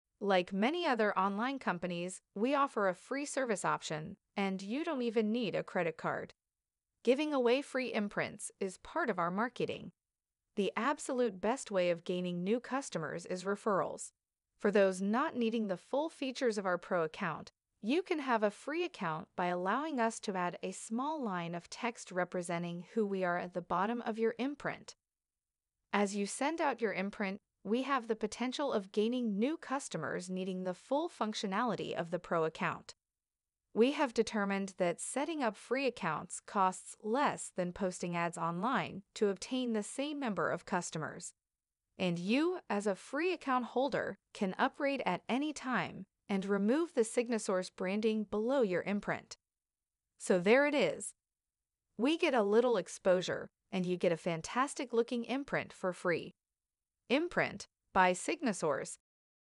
Free Custom Slideshow for Text and Email
For your text and email, a slide show of your products, services, testimonials, achievements, and more. Anything that describes who you are and what you do. Schedule future events, holidays, etc. This animated slideshow is placed directly into any text or email. A great way to captivate the reader and tell your story, so you don't have to. Proprietary apps to add the slideshow to your text as quickly as an emoji. Use our patented process to add the Imprint slideshow to your existing email signature, or take advantage of our included email signature creator. Updateable at any time, even previously sent emails.